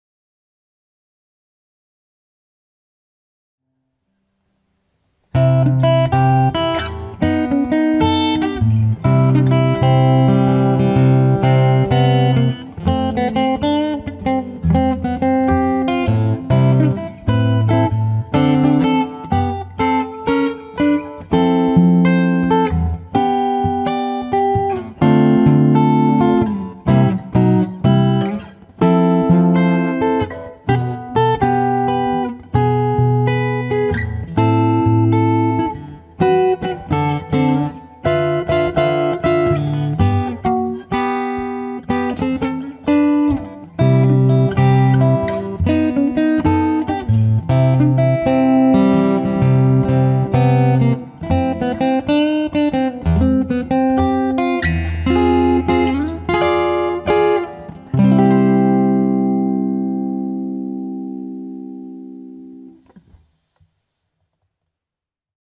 Active Melody - October 2020 Challenge (2nd submission) - Ragtime composition
Original composition by Clericus Vagans